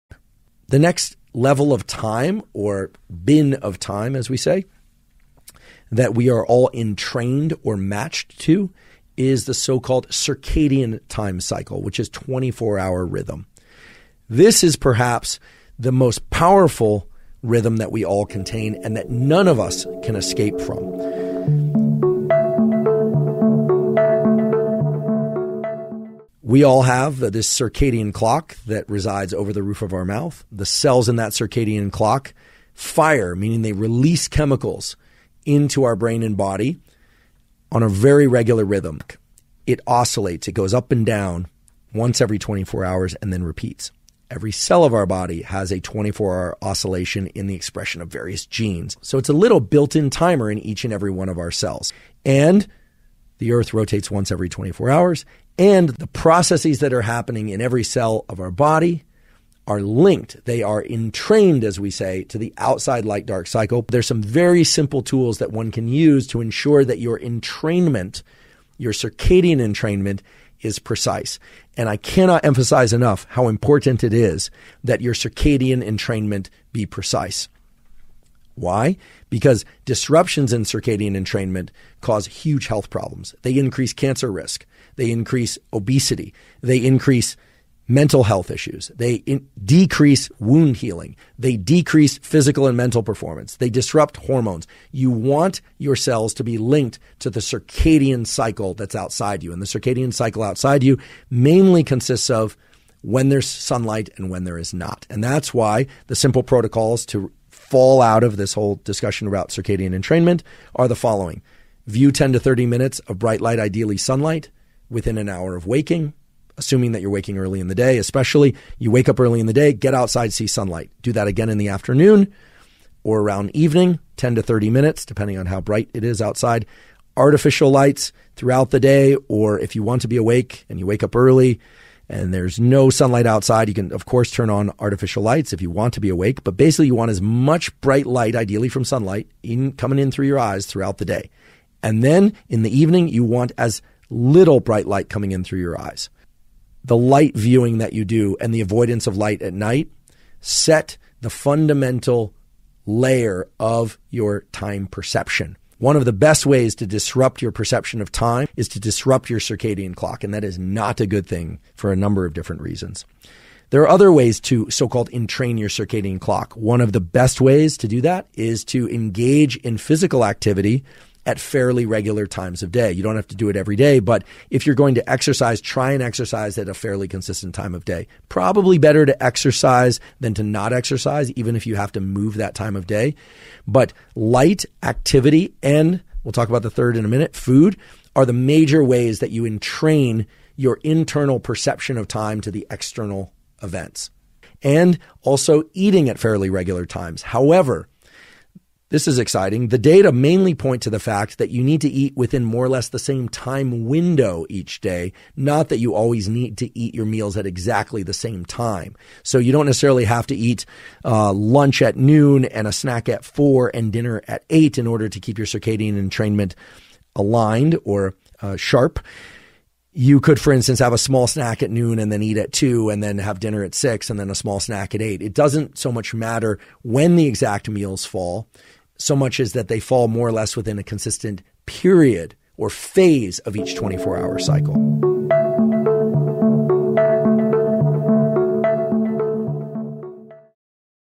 Timing the Mind | Andrew Huberman On Tuning Into the Circadian Rhythm
Dr. Andrew Huberman Reveals 3 ways you can tune into the Circadian Rhythm or Circadian Clock.

Timestamps
0:00 - Circadian Time Cycle
0:27 - The Circadian Clock
1:09 - Disrupting the Circadian Rhythm
1:50 - 3 Ways to Find the Circadian Rhythm

The original version of this audio has been edited in an attempt to compile some of its most relative segments. This version is an original creation and is not directly tied to the speaker. The full version is linked above.